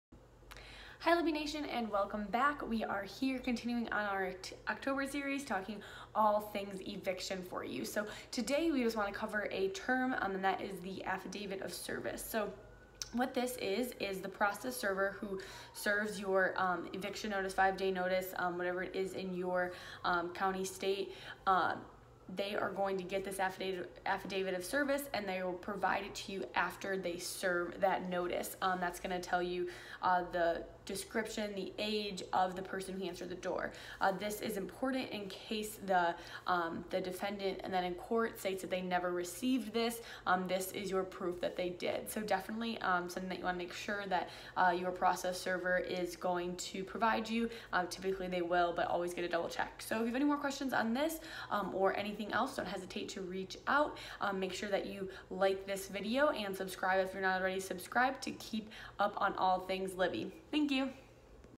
How To: Ensure Steps Aren't Overlooked During an Eviction
EvictionTips Be sure to always get a signed affidavit of service from your process server. This document proves that you did in fact have the legal documents served to the resident you'd like to evict & is necessary in court!